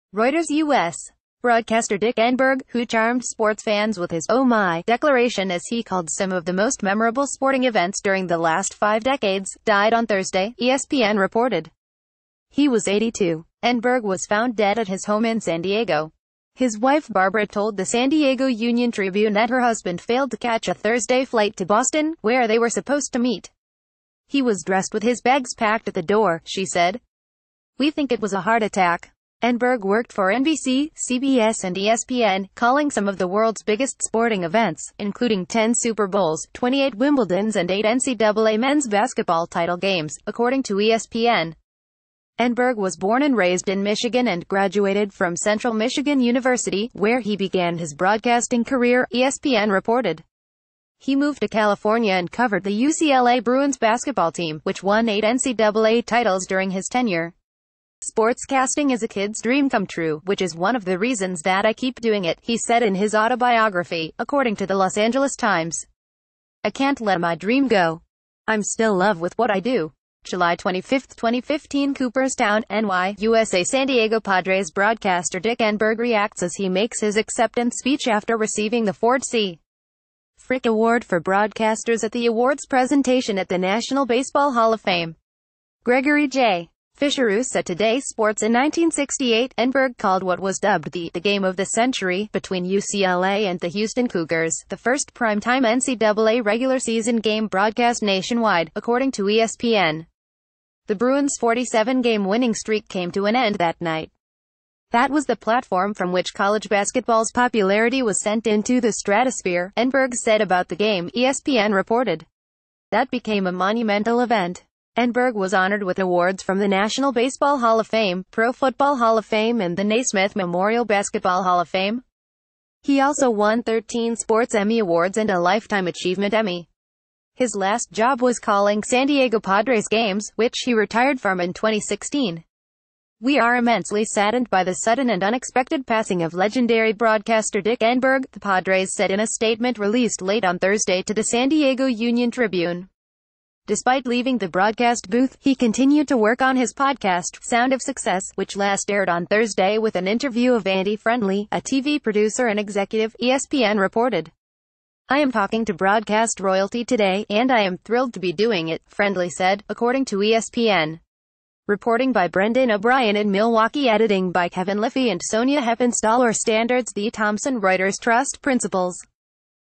TODAY NEWS - The US sports broadcaster dick enberg, 82, dies in san diego: media
TODAY NEWS - TODAY NEWS - The US sports broadcaster dick enberg, 82, dies in san diego: media

Reuters U.S. broadcaster Dick Enberg, who charmed sports fans with his “Oh My” declaration as he called some of the most memorable sporting events during the last five decades, died on Thursday, ESPN reported. He was 82. Enberg was found dead at his home in San Diego. His wife Barbara told the San Diego UnionTribune that her husband failed to catch a Thursday flight to Boston, where they were supposed to meet. “He was dressed with his bags packed at the door,” she said. “We think it was a heart ...